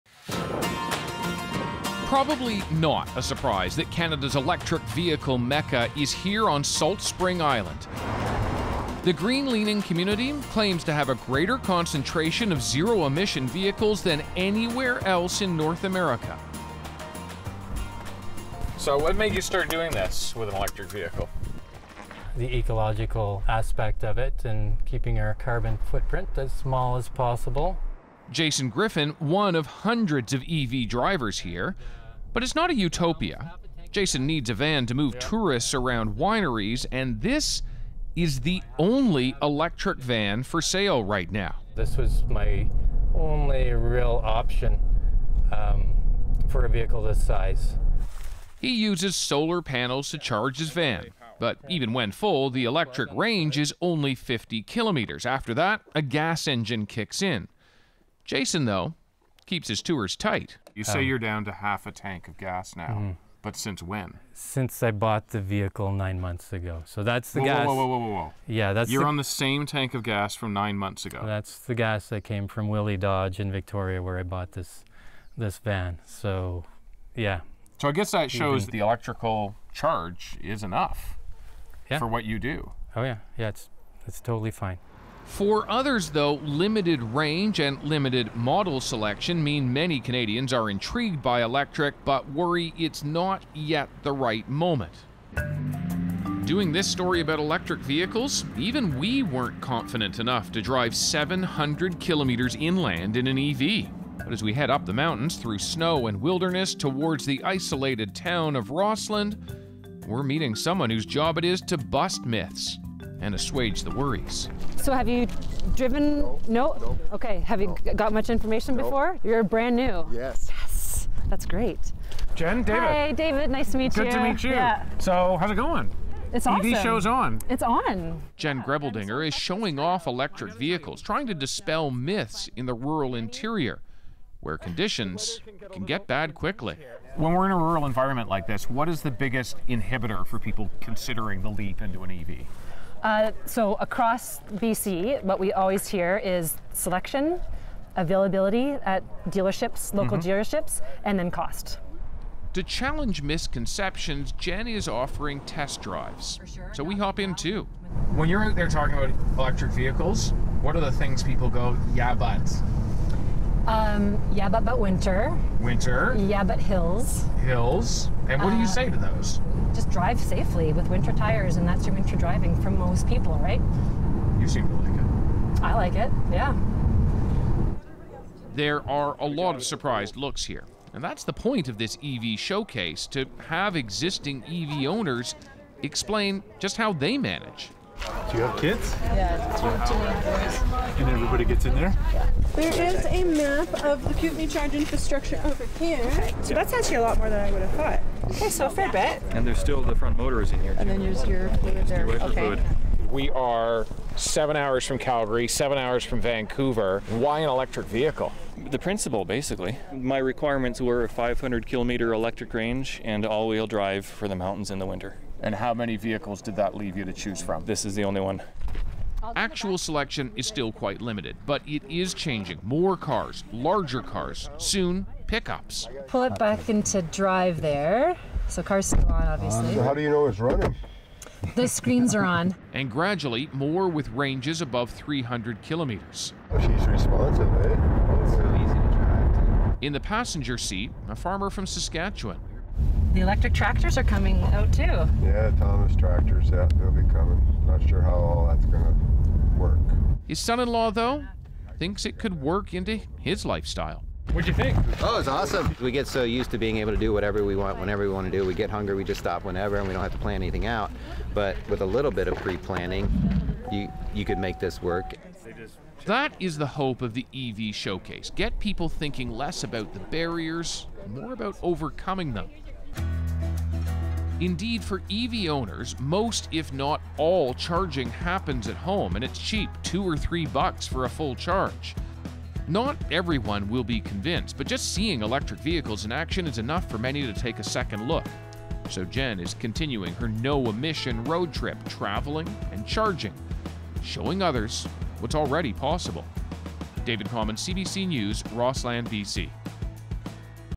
Are electric vehicles practical outside big cities?
The Canadian government wants drivers to go green, offering new federal rebates to help pay for it — but how realistic are EVs outside big cities? Where the distance between charging stations might be greater and road condition can be more challenging.